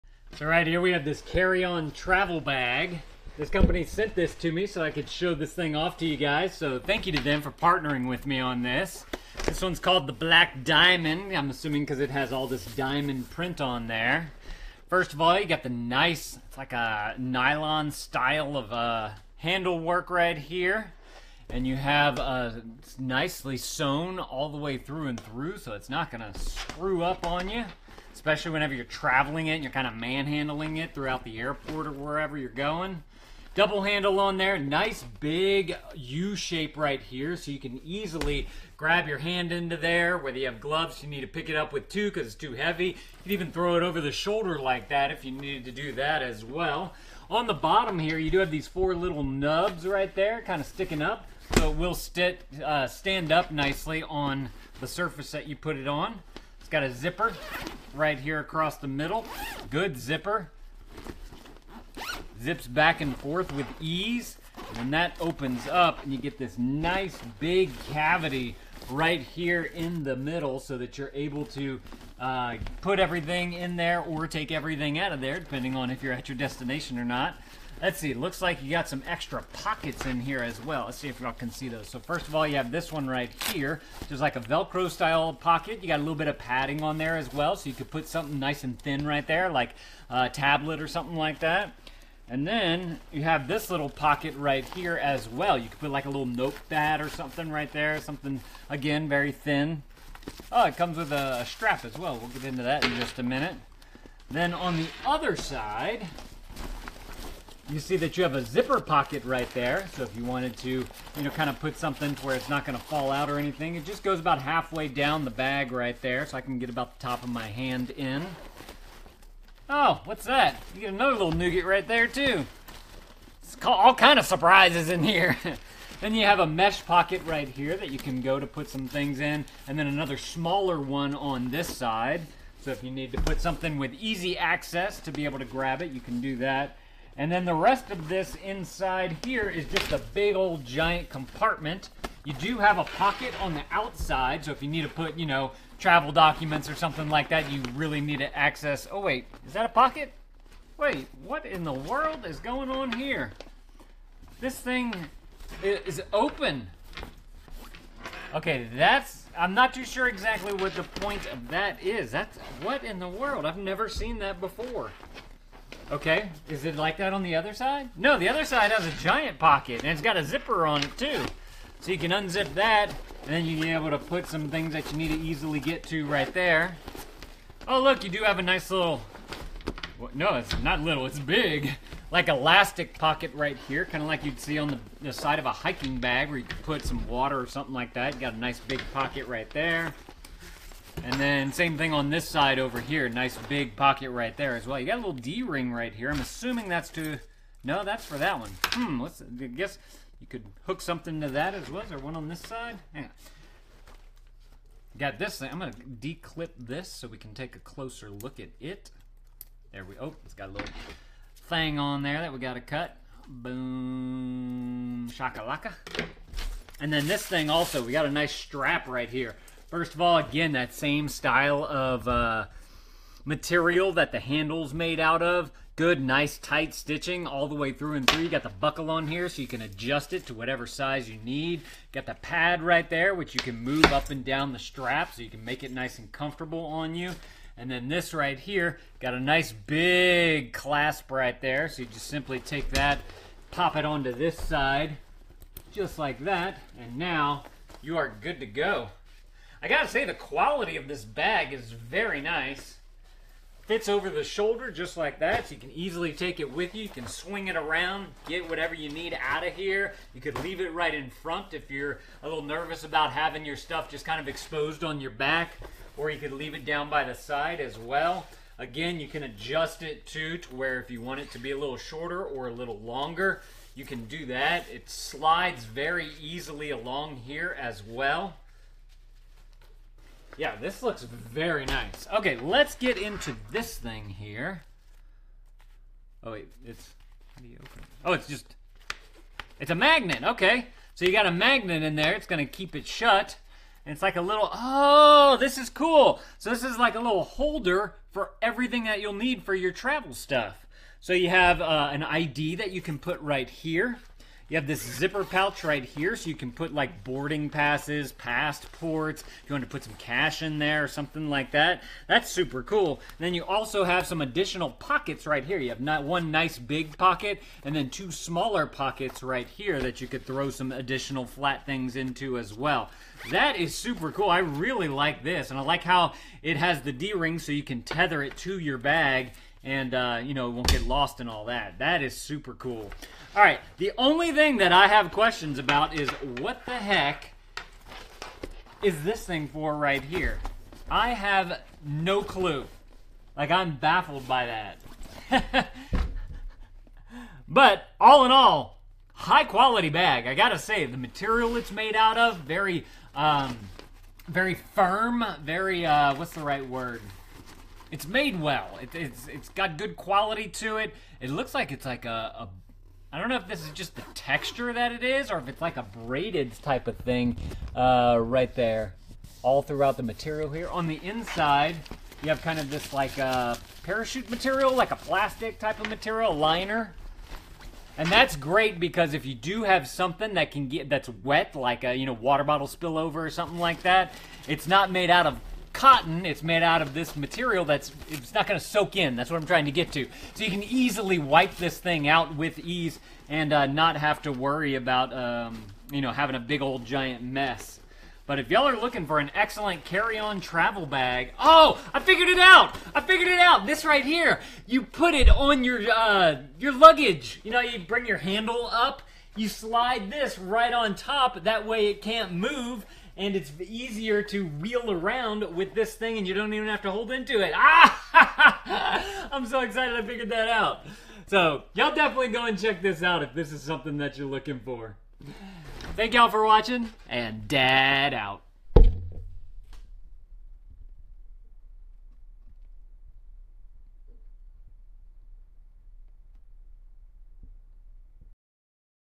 Best Secure Carry-On Travel Bags for 2023?
In this video, we're reviewing a carry-on travel bag with HIDDEN security features that was sent to me by the company. Called the Black Diamond, it has a sturdy nylon handle, extra pockets, solid stitching, and a magnetic travel organizer for your essentials. Perfect for travel.

00:00 - 00:22 Introduction to the Black Diamond Carry-On Bag
00:22 - 00:55 Bag Design and Handle Features
00:55 - 01:24 Internal Compartment and Pocket Overview
01:24 - 01:56 Additional Pockets and Organization
01:56 - 02:27 External Features and Zipper Pockets
02:27 - 03:01 Strap and Comfort Features
03:01 - 03:37 Travel Document Holder and Security Features
03:37 - 04:20 Quality and Material Discussion
04:20 - 05:00 Unique Features of the Bag
05:00 - 06:05 Final Thoughts and Recommendations
06:05 - 07:19 Conclusion and Call to Action

Do you want your product featured on Dad Reviews?